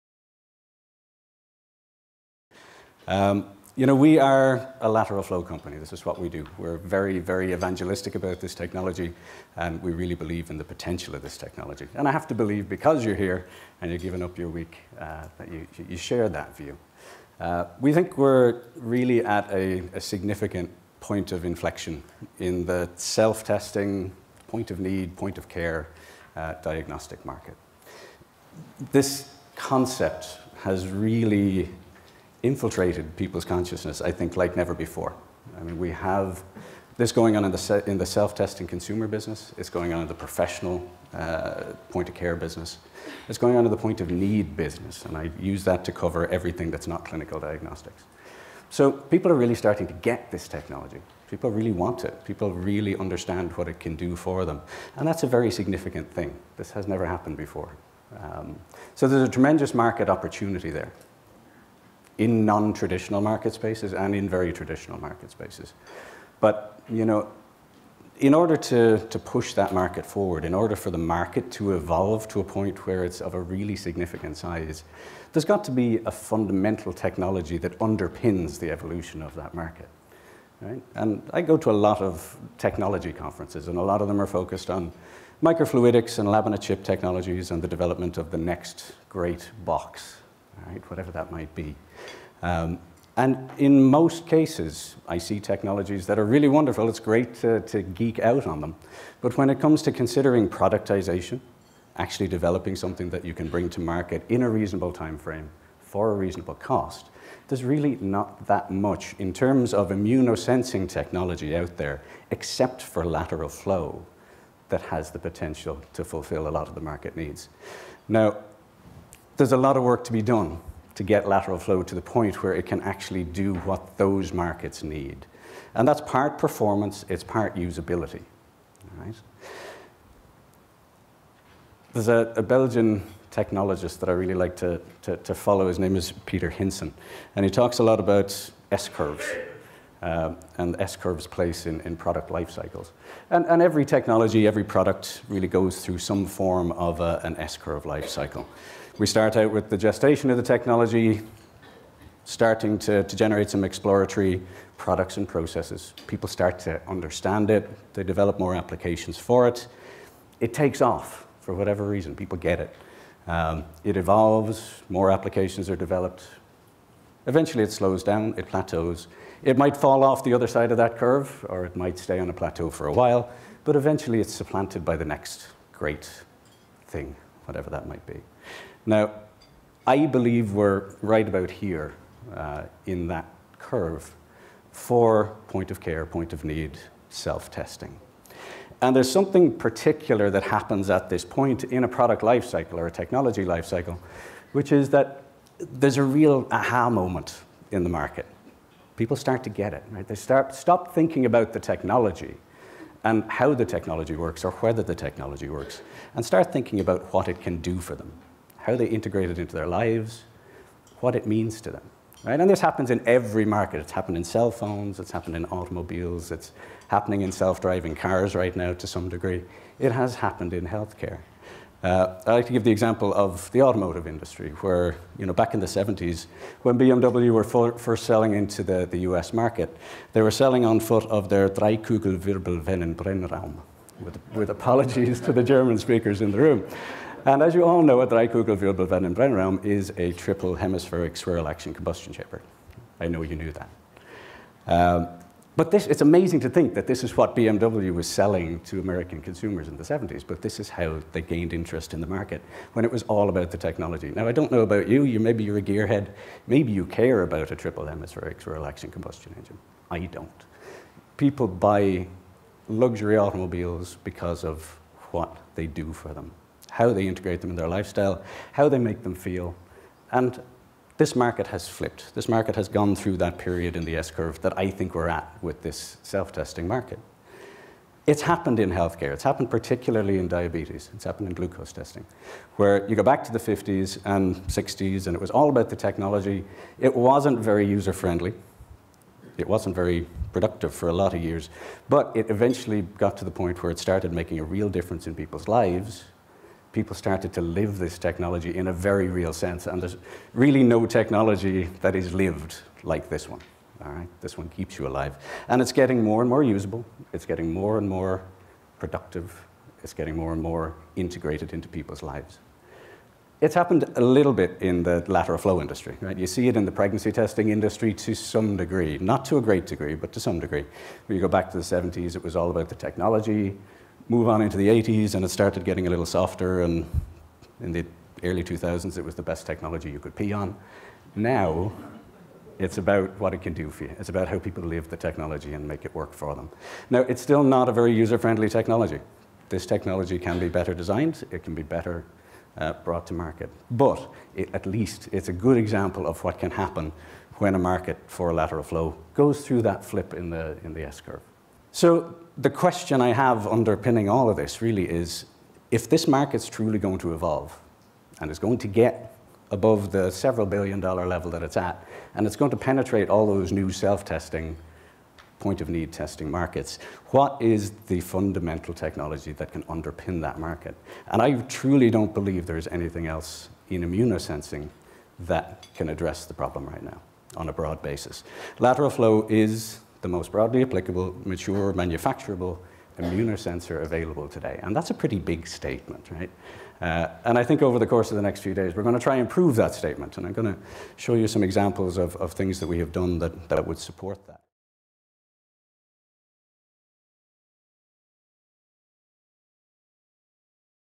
ALFC 2017 Teaser- The Evolution in Lateral Flow
The 4th annual Advanced Lateral Flow Course will be taking place in San Diego this October. Over the course of 3 days, we will review all aspects of rapid test design, development, manufacturing and commercialization. This video is a short introduction to the course, and provides insight into how the lateral flow technology has evolved, and where it seems to be heading.

Register today to learn more about the evolution of lateral flow technology and how to enter the market with your own rapid test design.